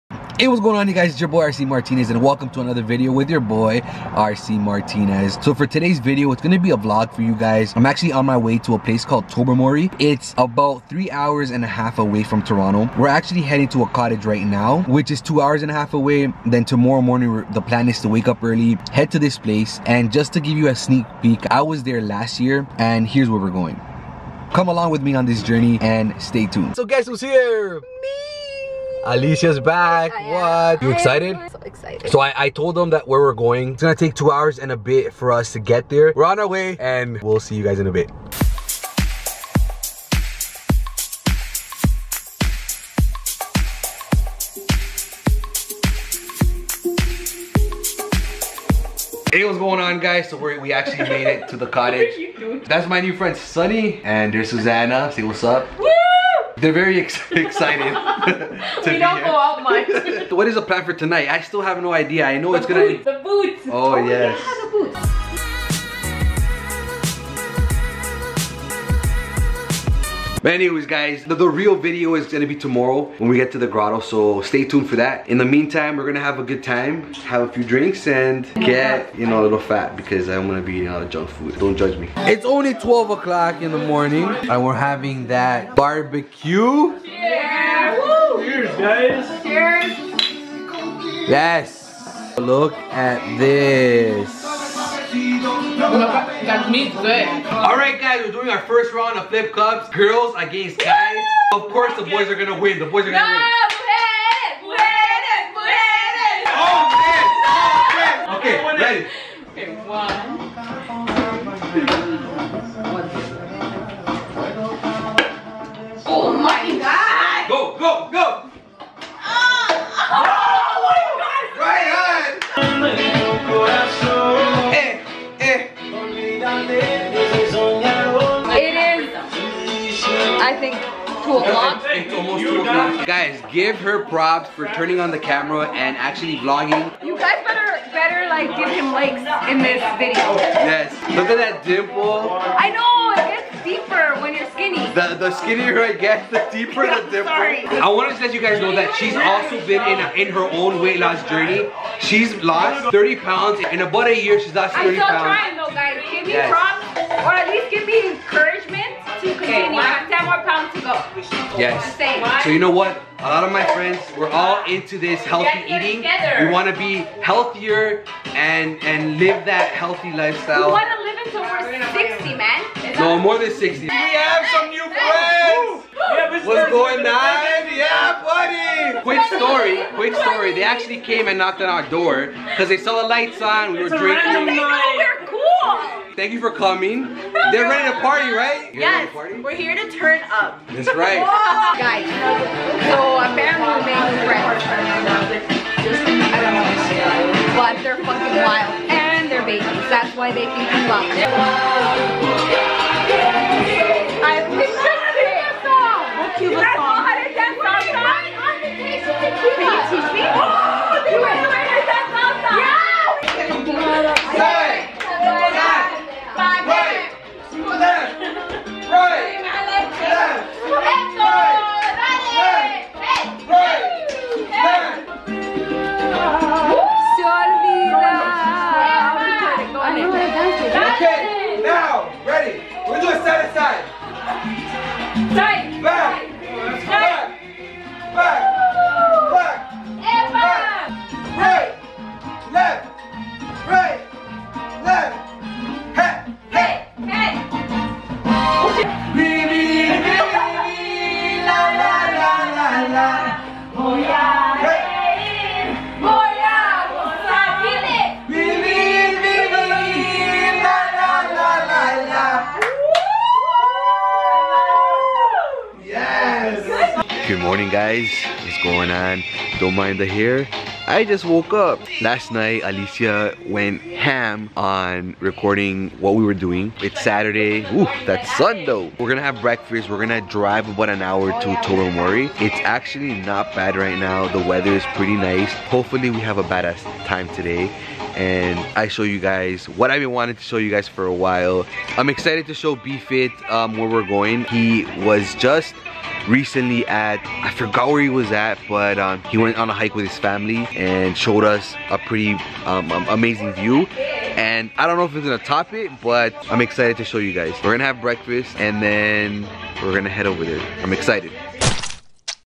Trip to Tobermory Pt1 SALSA DANCING Vlog 24 | RCMrtnz
Twitter and IG: @RCMrtnz

July will be the month of vlogs!
On this video, we head to Tobermory, Bruce Peninsula. Well our cottage was in Saugeen Shores which was 1.5 hours away. And that was out first night there haha
Watch till the end for the salsa dancing!
Next week will be PART 2! You don't want to miss it!

Tobermory
is a harbour village on the Bruce Peninsula in the province of Ontario. Just southeast, Bruce Peninsula National Park is known for the Bruce Trail footpath on the craggy Niagara Escarpment, and the Grotto, a wave-carved cave in Georgian Bay. The village lies next to Fathom Five National Marine Park, which is home to the Flowerpot Island sea stacks, 19th-century lighthouses and multiple shipwreck dive sites.

Cheers, RC-